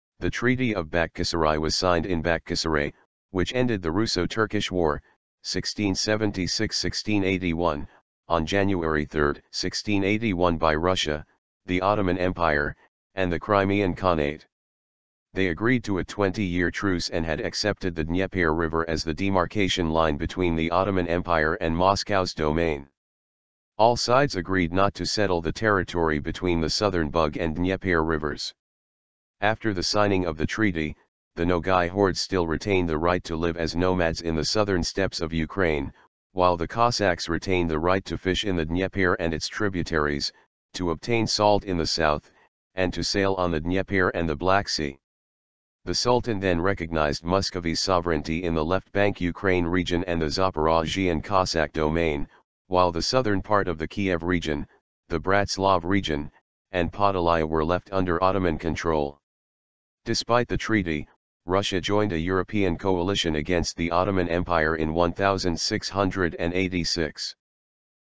The Treaty of Bakhchisarai
The Treaty of Bakhchisarai was signed in Bakhchisaray, which ended the Russo-Turkish War (1676–1681), on 3 January 1681 by Russia, the Ottoman Empire, and the Crimean Khanate. They agreed to a 20-year truce and had accepted the Dnieper River as the demarcation line between the Ottoman Empire and Moscow's domain. All sides agreed not to settle the territory between the Southern Bug and Dnieper rivers. After the signing of the treaty, the Nogai hordes still retained the right to live as nomads in the southern steppes of Ukraine, while the Cossacks retained the right to fish in the Dnieper and its tributaries; to obtain salt in the south; and to sail on the Dnieper and the Black Sea. The sultan then recognized Muscovy's sovereignty in the Left-bank Ukraine region and the Zaporozhian Cossack domain, while the southern part of the Kiev region, the Bratslav region, and Podolia were left under Ottoman control.

Despite the treaty, Russia joined a European coalition against the Ottoman Empire in 1686.